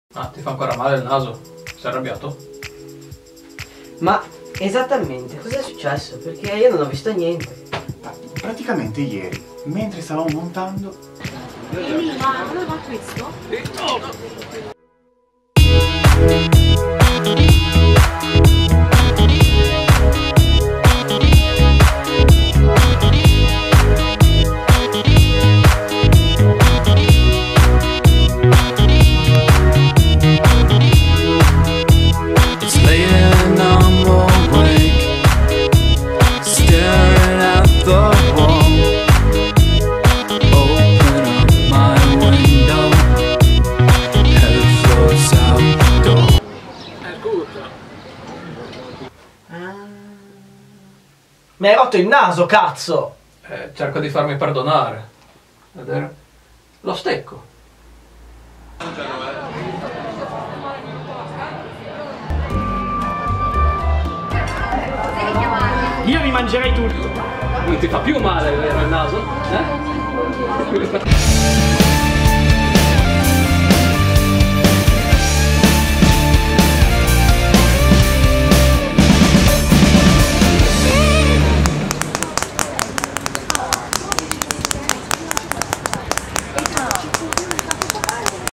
La sala prove - ep. 10 - Danni collaterali
Ecco cosa succede nella nostra salaprove quando troviamo una data! Vi aspettiamo da "Lo Stecco" il 9/07/2018 in zona porto di Loano!!